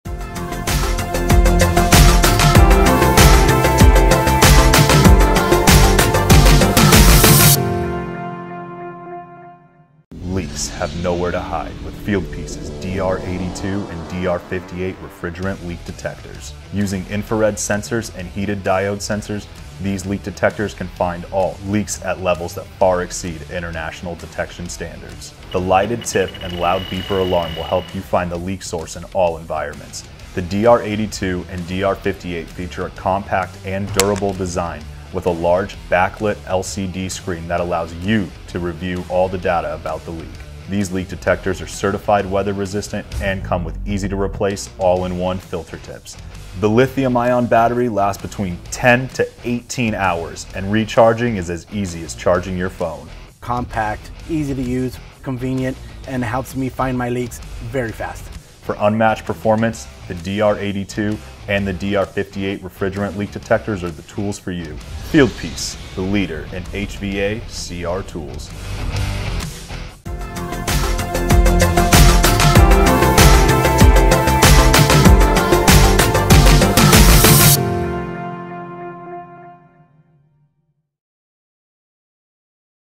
Fieldpiece Refrigerant Leak Detector - Two Best Models DR82 and DR58
We encourage everyone to watch the video Fieldpiece Refrigerant Leak Detector - Two Best Models Fieldpiece DR82 and Fieldpiece DR58. Learn more, shop, and get a Deal at Value Testers

These Fieldpiece refrigerant leak detectors feature a higher level of performance and offer new, bright blue backlit LCD screens that are easy to read.

Fieldpiece's DR82 and DR58 are designed for the field with rugged, impact- and water-resistant, and are IP54-certified. Each leak detector also comes with a blow-molded carrying case.

The Fieldpiece DR82 and Fieldpiece DR58 will make your job faster and easier.